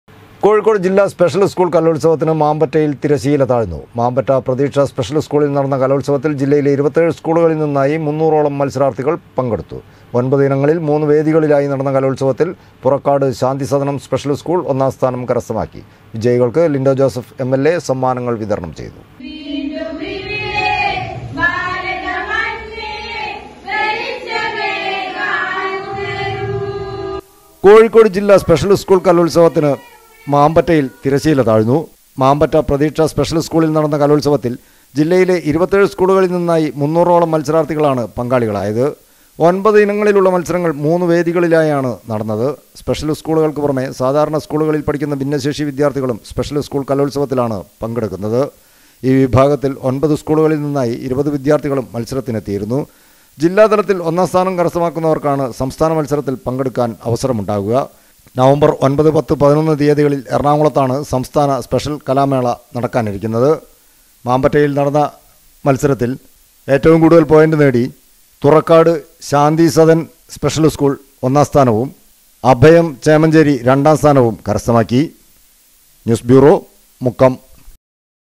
കോഴിക്കോട് ജില്ലാ സ്പെഷ്യൽ സ്കൂൾ കലോത്സവത്തിന് മാമ്പറ്റയിൽ തിരശ്ശീല താഴ്ന്നു.
കോഴിക്കോട് ജില്ലാ സ്പെഷ്യൽ സ്കൂൾ കലോത്സവത്തിന് മാമ്പറ്റയിൽ തിരശ്ശീല താഴ്ന്നു. മാമ്പറ്റ പ്രതീക്ഷ സ്പെഷ്യൽ സ്കൂളിൽ നടന്ന   കലോത്സവത്തിൽ ജില്ലയിലെ 27 സ്കൂളുകളിൽ നിന്നായി മുന്നൂറോളം മത്സരാർഥികൾ പങ്കെടുത്തു.